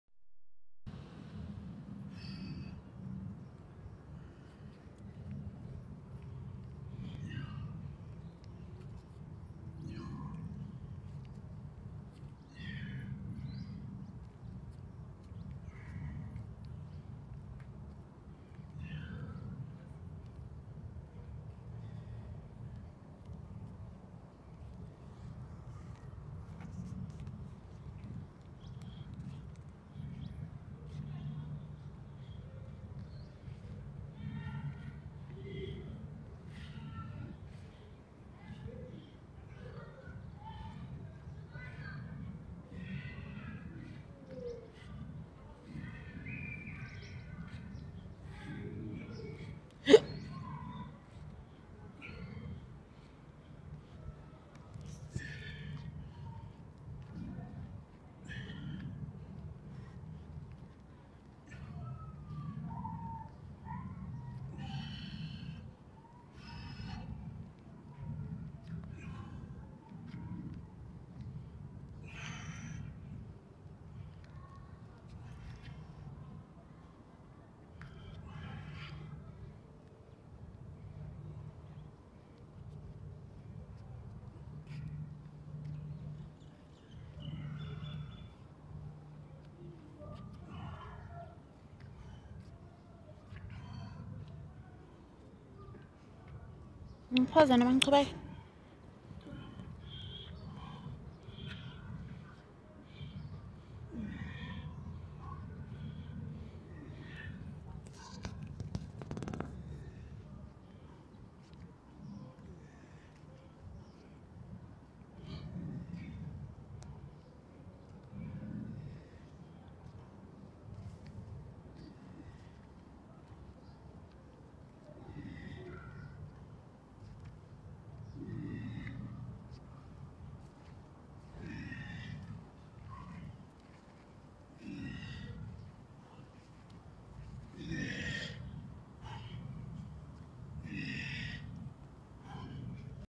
Two minutes warm up squad. healthy squad training Ronnie Coleman, how I do it
This exercise help me to increase my legs muscles. I'm at the heights Pretoria West preparing for tomorrow's hard Training